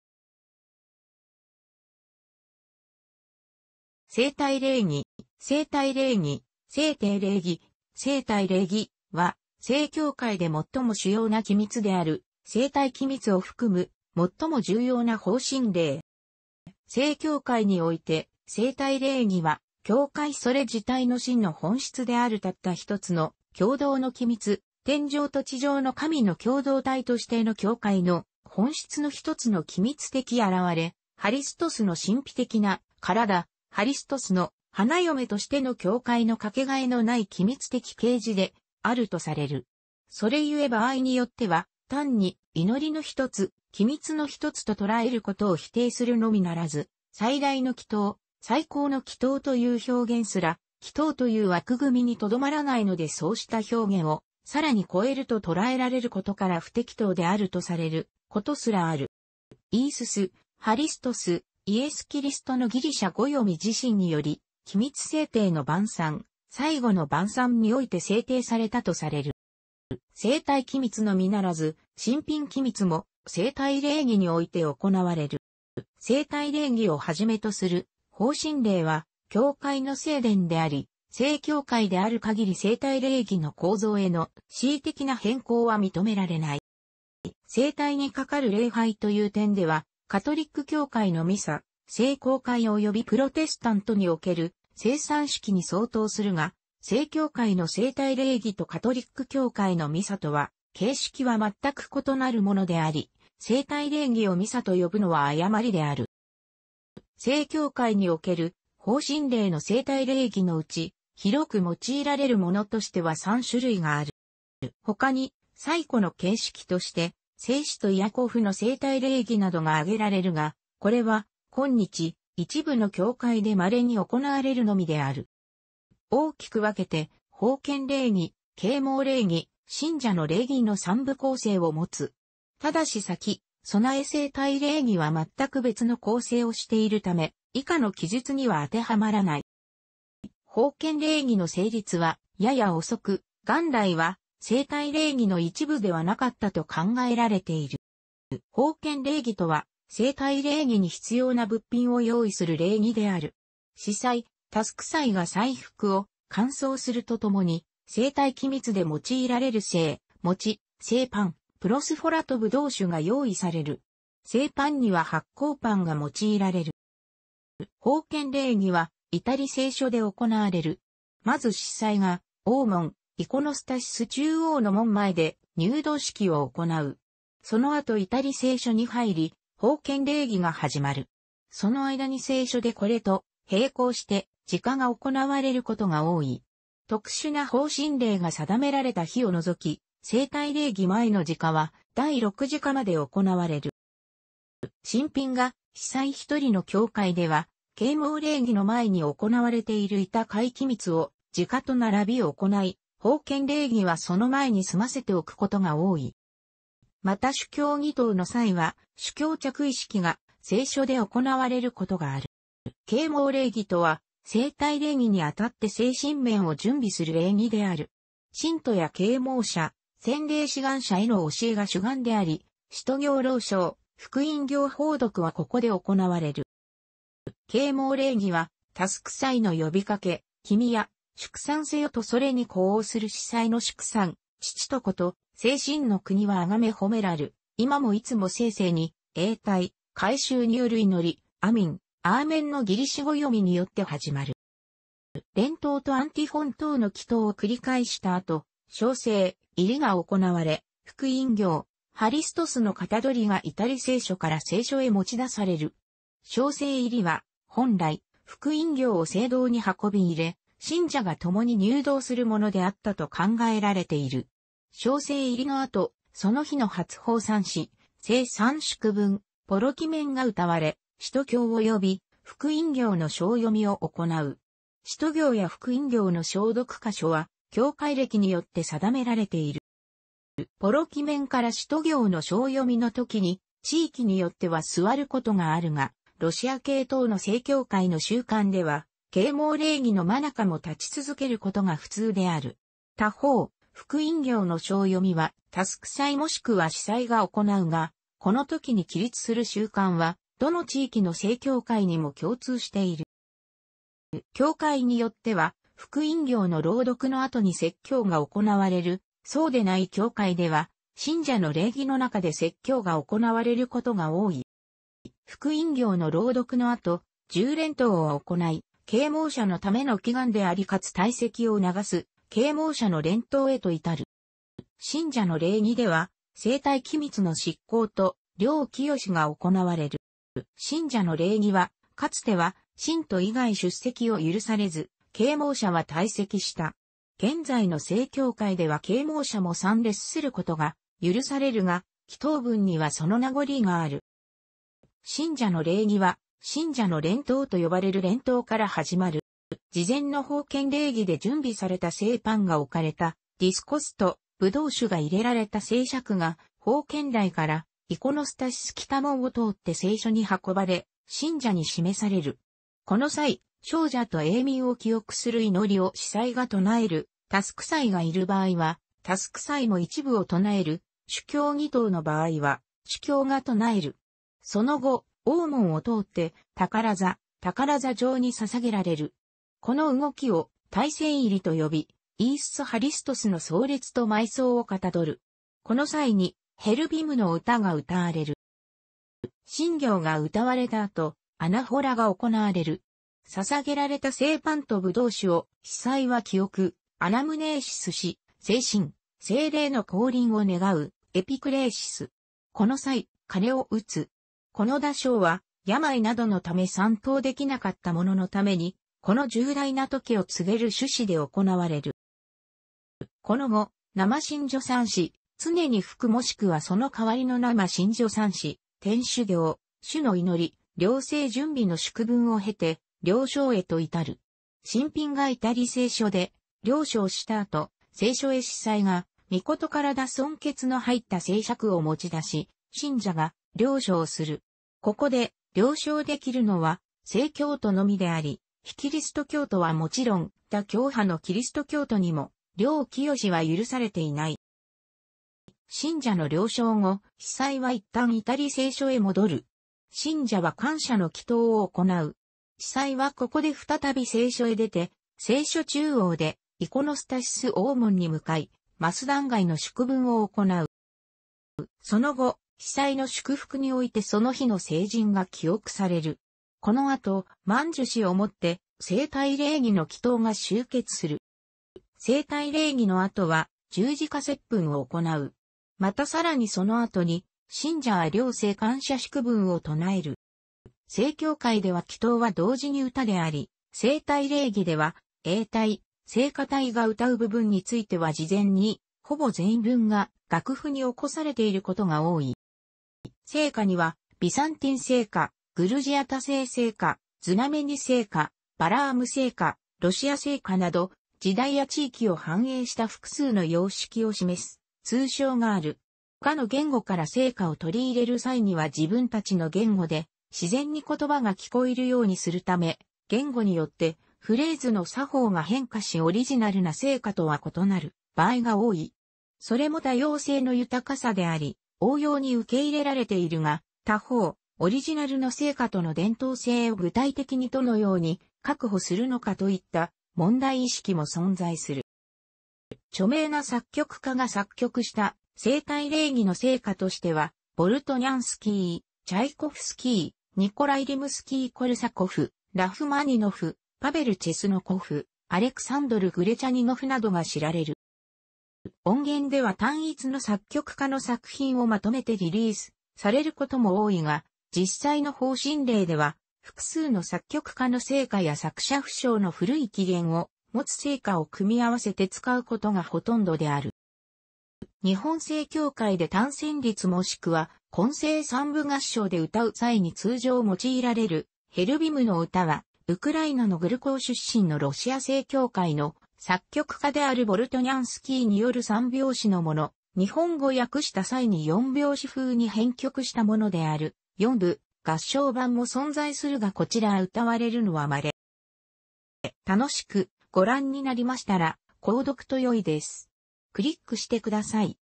聖体礼儀
聖体礼儀（聖體禮儀、せいたいれいぎ、, , ）は正教会で最も主要な機密である聖体機密を含む、最も重要な奉神礼。

正教会において聖体礼儀は、「教会それ自体の真の本質であるたったひとつの共同の機密」「天上と地上の神の共同体としての教会の本質の一つの機密的現れ」「ハリストスの神秘的な『からだ』」「ハリストスの『花嫁』としての教会のかけがえのない機密的啓示」であるとされる。それゆえ場合によっては、単に「祈りの一つ」「機密の一つ」と捉えることを否定するのみならず、「最大の祈祷」「最高の祈祷」という表現すら（祈祷という枠組みにとどまらないのでそうした表現をさらに越えると捉えられることから）不適当であるとされることすらある。

イイスス・ハリストス（イエス・キリストのギリシャ語読み）自身により、機密制定の晩餐（最後の晩餐）において制定されたとされる。

聖体機密のみならず神品機密も聖体礼儀において行われる。

聖体礼儀をはじめとする奉神礼は教会の聖伝であり、正教会である限り聖体礼儀の構造への恣意的な変更は認められない。

聖体にかかる礼拝という点ではカトリック教会のミサ、聖公会およびプロテスタントにおける聖餐式に相当するが、正教会の聖体礼儀とカトリック教会のミサとは形式は全く異なるものであり、聖体礼儀をミサと呼ぶのは誤りである。

正教会における奉神礼の聖体礼儀のうち、広く用いられるものとしては3種類がある。

他に最古の形式として聖使徒イアコフの聖体礼儀などが挙げられるが、これはこんにち、一部の教会で稀に行われるのみである。

大きく分けて、「奉献礼儀・啓蒙礼儀・信者の礼儀」の三部構成をもつ。ただし先備聖体礼儀は全く別の構成をしているため、以下の記述には当て嵌まらない。

奉献礼儀の成立はやや遅く、元来は聖体礼儀の一部ではなかったと考えられている。

奉献礼儀（ほうけんれいぎ）とは、聖体礼儀に必要な物品を用意する礼儀である。司祭・輔祭が祭服を完装するとともに、聖体機密で用いられる聖餅（聖パン、プロスフォラ）とぶどう酒が用意される。聖パンには発酵パンが用いられる。

奉献礼儀は至聖所で行われる。まず司祭が王門（イコノスタシス中央の門）前で入堂式を行う。そのあと至聖所に入り、奉献礼儀がはじまる。その間に聖所でこれと平行して時課が行われることが多い。特殊な奉神礼が定められた日を除き、聖体礼儀前の時課は第六時課まで行われる。

神品が司祭一人の教会では、啓蒙礼儀の前に行なわれている痛悔機密を時課と並び行ない、奉献礼儀はその前に済ませておくことが多い。

また主教祈祷の際は、主教着衣式が、聖所で行われることがある。

啓蒙礼儀（けいもうれいぎ）とは、聖体礼儀にあたって精神面を準備する礼儀である。信徒や啓蒙者（洗礼志願者）への教えが主眼であり、使徒経朗誦、福音経奉読はここで行われる。

啓蒙礼儀は、輔祭の呼びかけ「君や、祝讃せよ」とそれに呼応する司祭の祝讃「父と子と聖神の国は崇め讃めらる、今もいつも世々に」、詠隊（会衆）による祈り「アミン」（アーメンのギリシャ語読み）によって始まる。

連祷とアンティフォン等の祈祷を繰り返した後、小聖入が行われ、福音経（ハリストスの象り）が至聖所から聖所へ持ち出される。小聖入は本来、福音経を聖堂に運び入れ、信者が共に入堂するものであったと考えられている。小聖入のあと、その日の発放讃詞、聖三祝文、ポロキメンが歌われ、使徒経および福音経の誦読を行う。使徒経や福音経の誦読箇所は、教会暦によって定められている。

ポロキメンから使徒経の誦読のときに、地域によっては座ることがあるが、ロシア系等の正教会の習慣では、啓蒙礼儀の間中も立ち続けることが普通である。他方、福音経の誦読は輔祭もしくは司祭が行うが、このときに起立する習慣はどの地域の正教会にも共通している。

教会によっては福音経の朗読のあとに説教が行われる（そうでない教会では信者の礼儀のなかで説教が行われることが多い）。

福音経の朗読のあと、重連祷（じゅうれんとう）を行い、啓蒙者のための祈願でありかつ退席を促す「啓蒙者の連祷」へと到る。

信者の礼儀（しんじゃのれいぎ）では、聖体機密の執行と領聖が行われる。

信者の礼儀は、かつては信徒以外出席を許されず、啓蒙者は退席した。現在の正教会では啓蒙者も参列することが許されるが、祈祷文にはその名残がある。

信者の礼儀は、「信者の連祷」と呼ばれる連祷から始まる。

事前の奉献礼儀で準備された聖パンが置かれたディスコスと、...